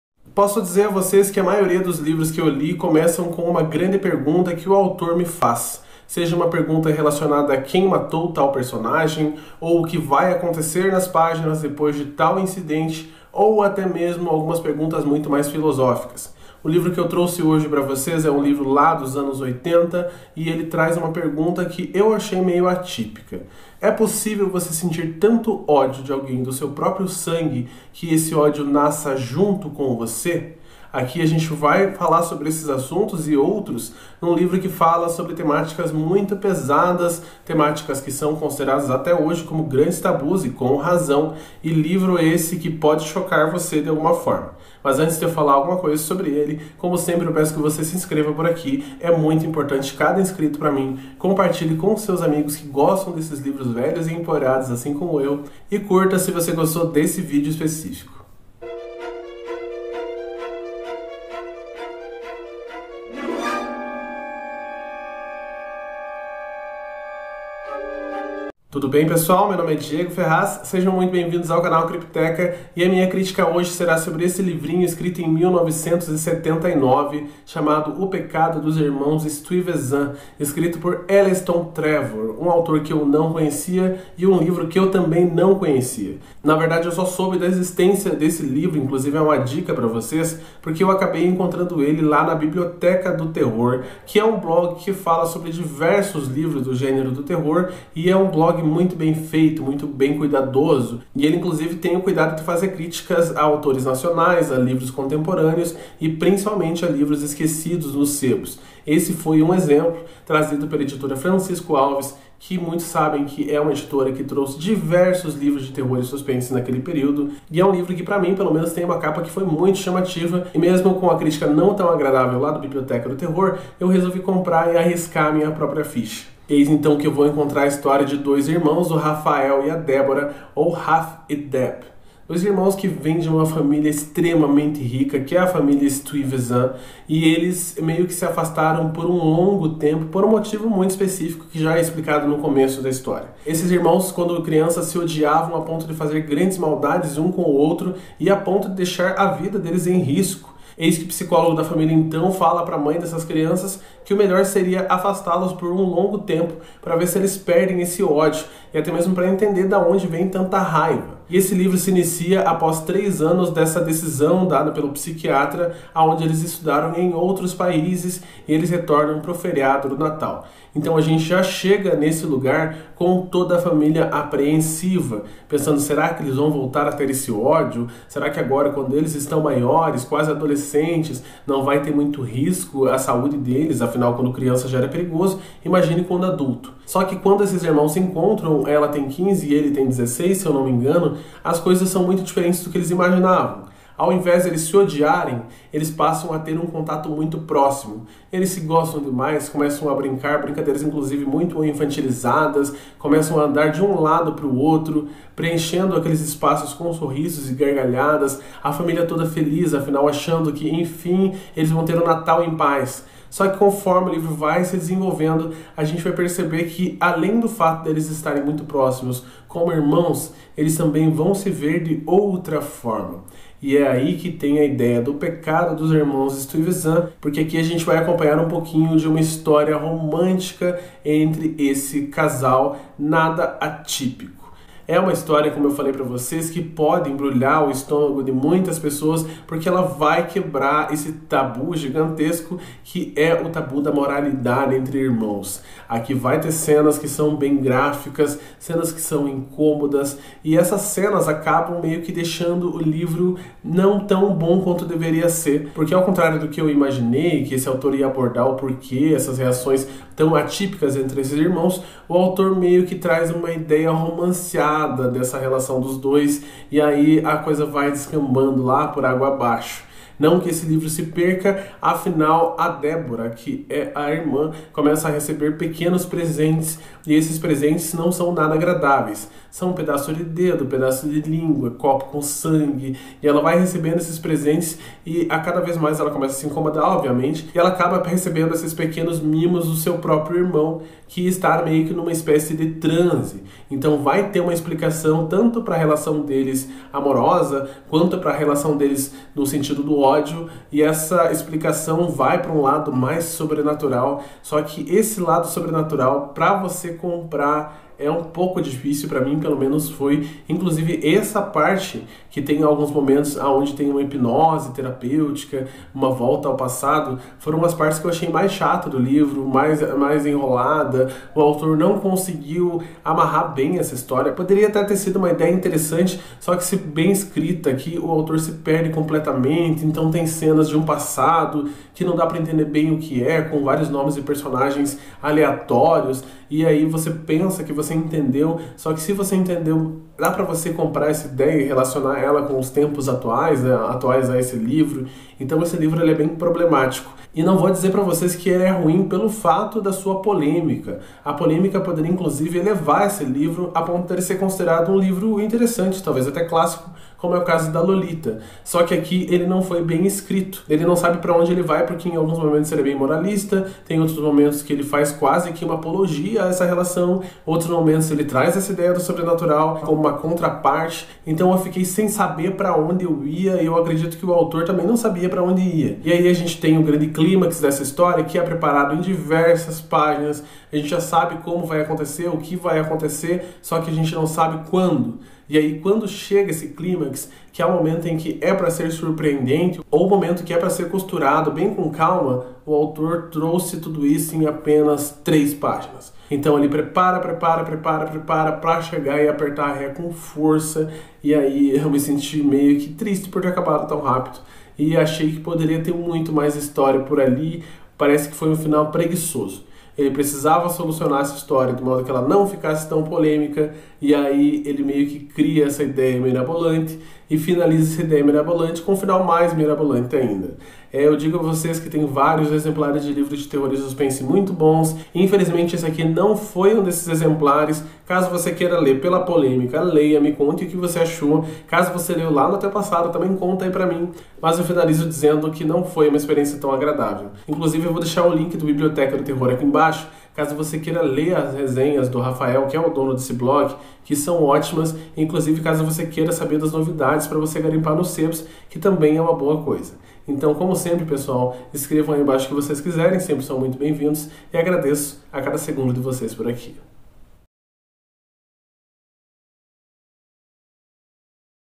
O PECADO DOS IRMÃOS STUYVESSANT - ELLESTON TREVOR -O QUE ELES ESCONDEM?
Dois irmãos que se odeiam quando crianças passam a ser perigosos um para o outro. Eis que, convencidos por um psicólogo, a família resolve os afastar por três anos. No reencontro dos irmãos, em meio a um cenário de apreensão de todos há uma surpresa generalizada quando eles passam a se dar muito bem, como se nada tivesse acontecido no passado. Será que o ódio realmente deixou de existir? Ou será que estamos vendo apenas uma suposta paz que antecede grandes tragédias?